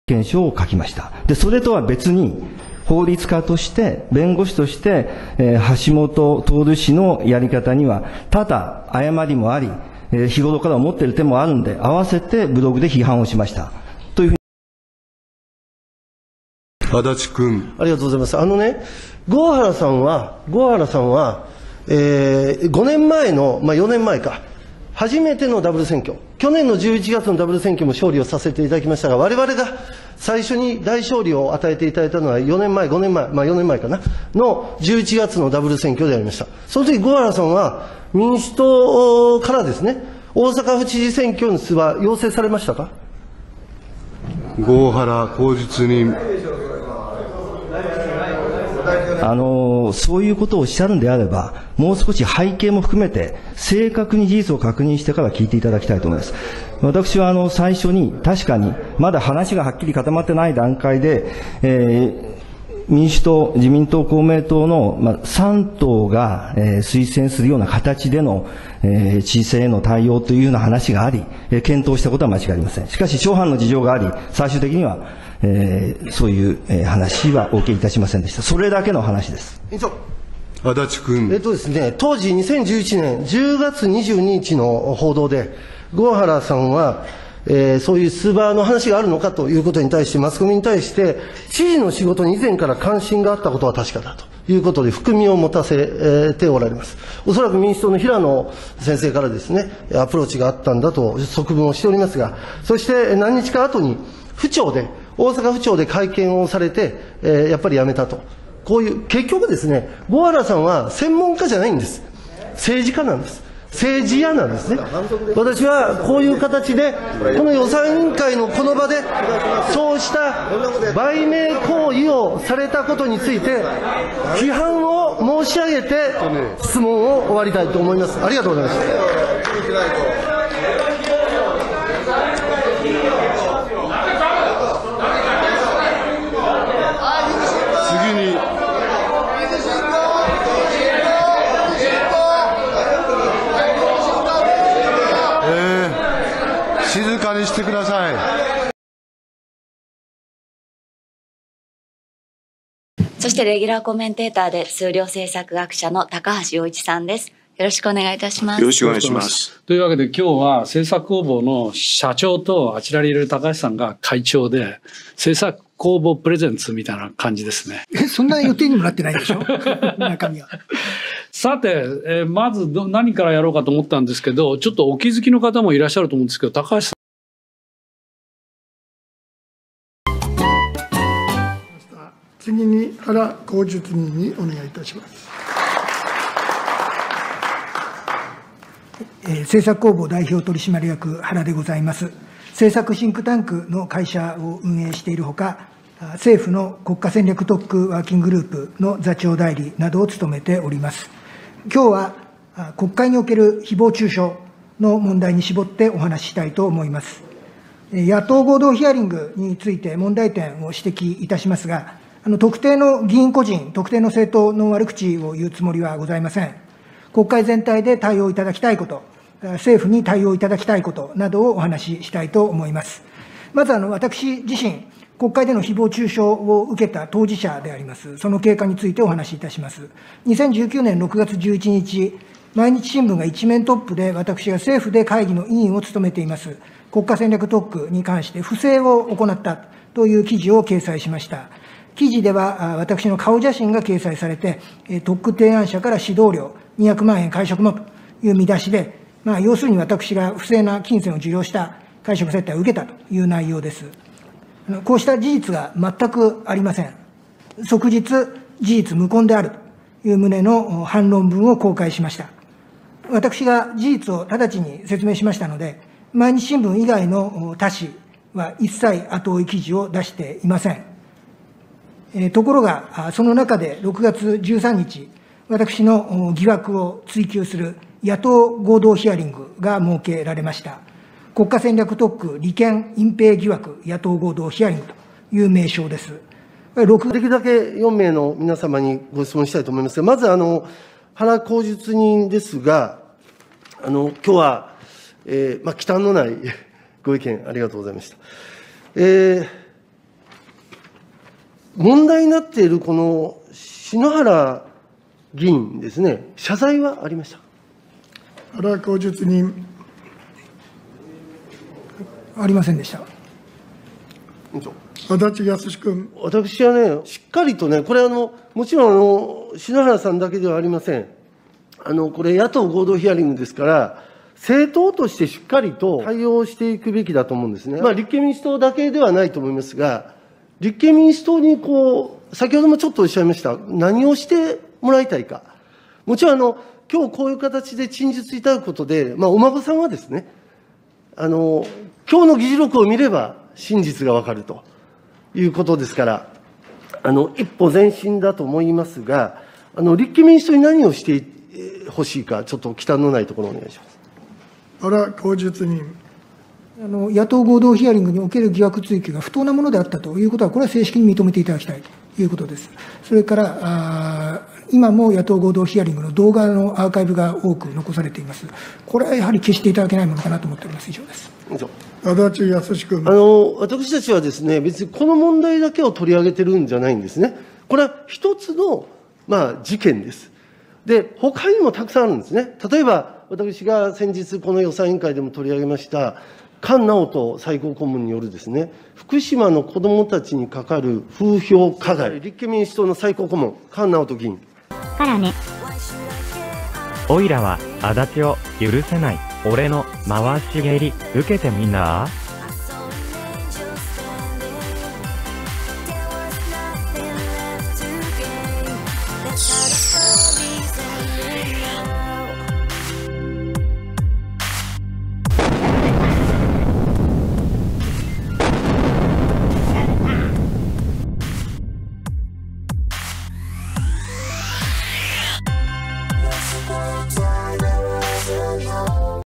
足立康史、暴虐行為止まらず。郷原信郎氏に対する無礼、 時計泥棒 高橋洋一のグル原英史に立憲民主党無礼の強要。足立に憤慨の少年少女。
何度注意されても止めない足立。創価学会の罪人遠山清彦にさえ注意された。